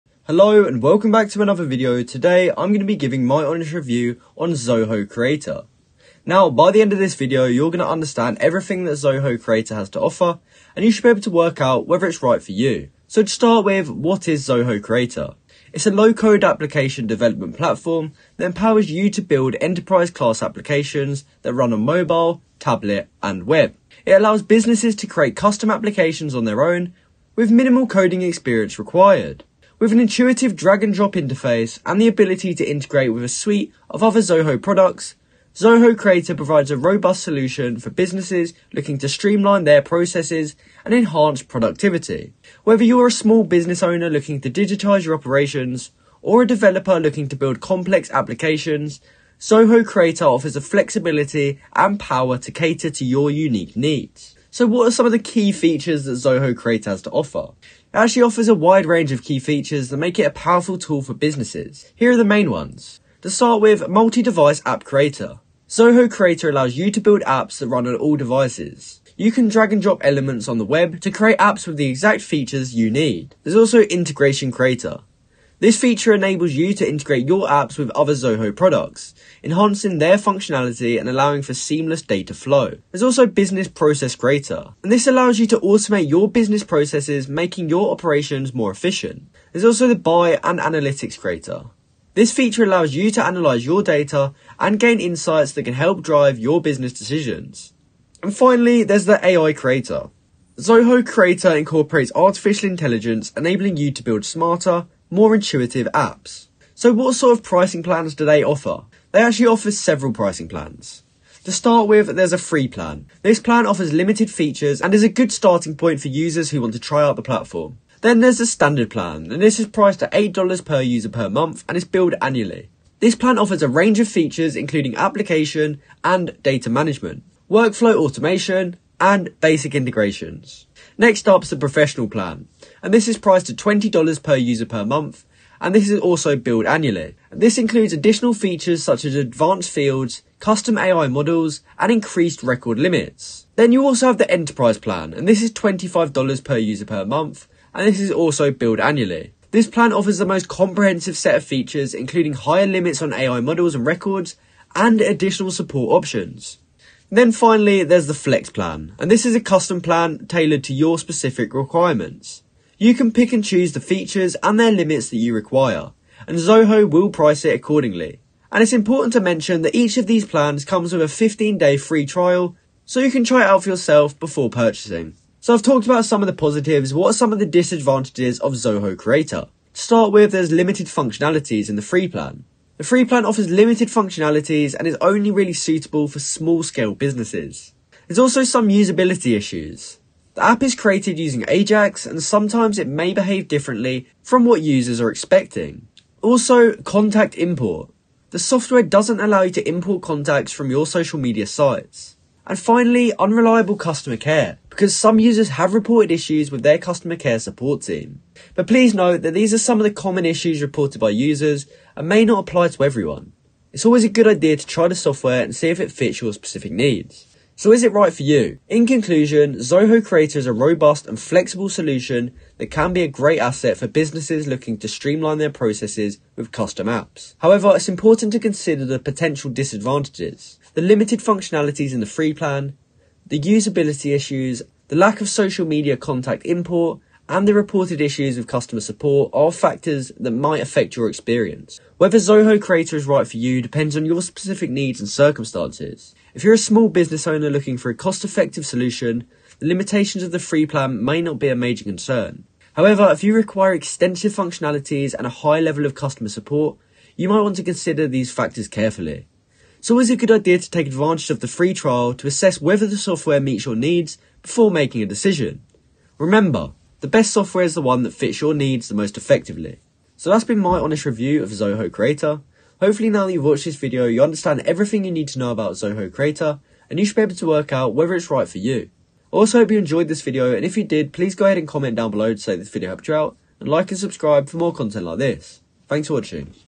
Zoho Creator Review: Is It Right For You? (2025)
In today's video i will give my honest review on zoho creator!